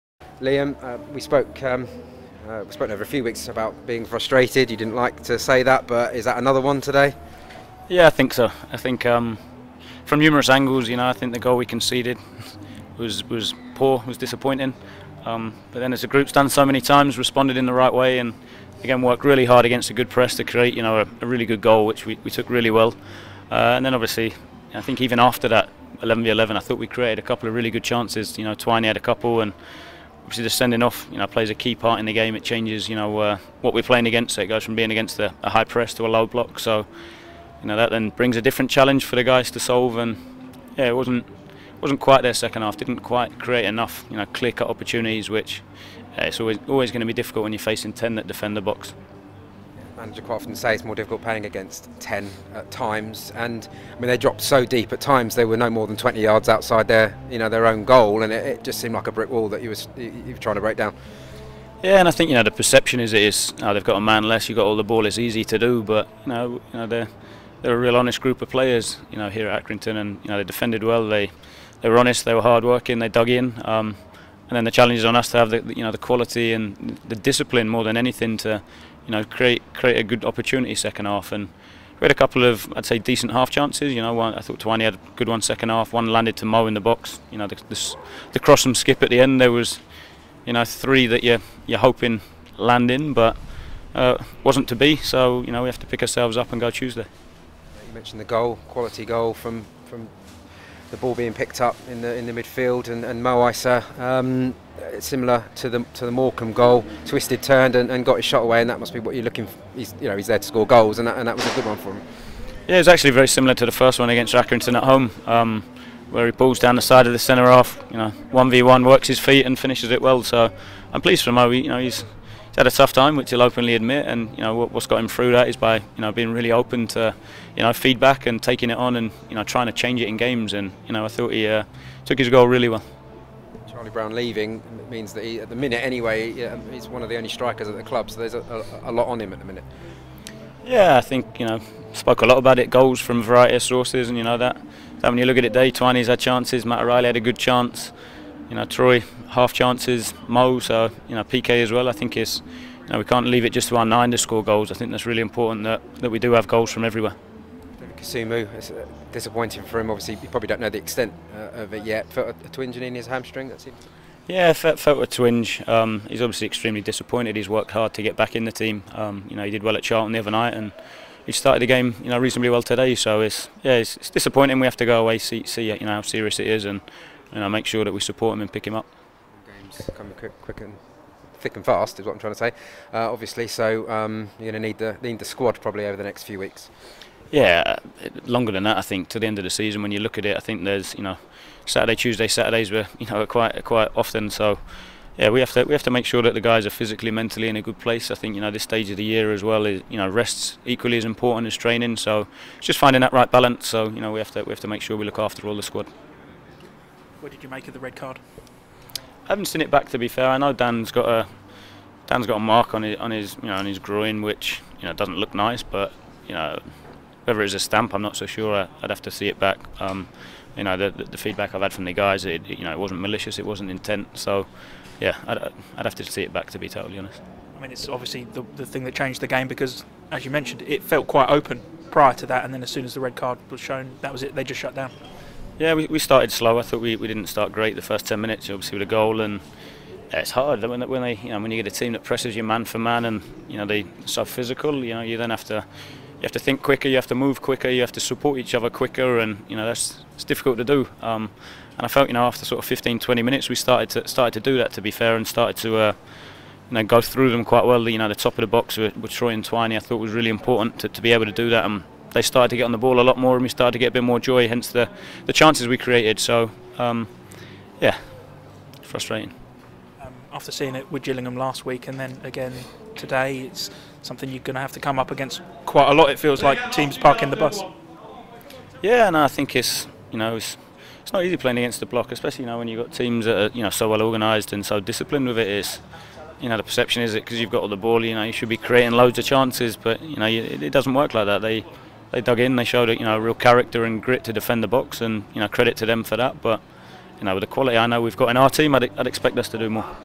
PRESS CONFERENCE: Liam Manning's Accrington Stanley reaction
Head Coach Liam Manning speaks to the media following his side's 1-1 draw with Accrington Stanley...